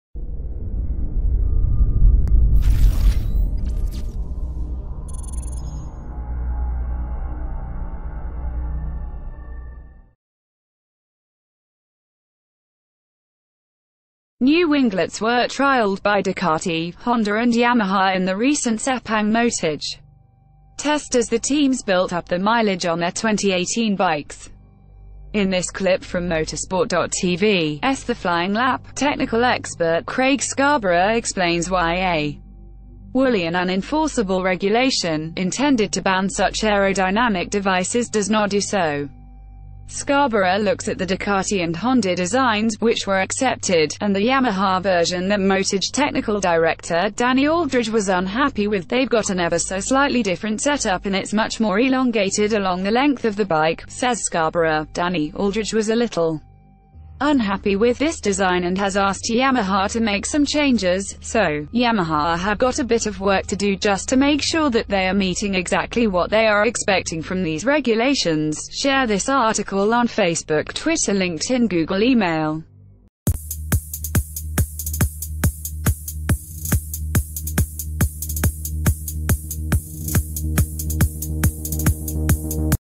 MotoGPs fresh winglet controversy with 2018 bikes explained | by Automobiles
► New winglets were trialled by Ducati, Honda and Yamaha in the recent Sepang MotoGP test as the teams...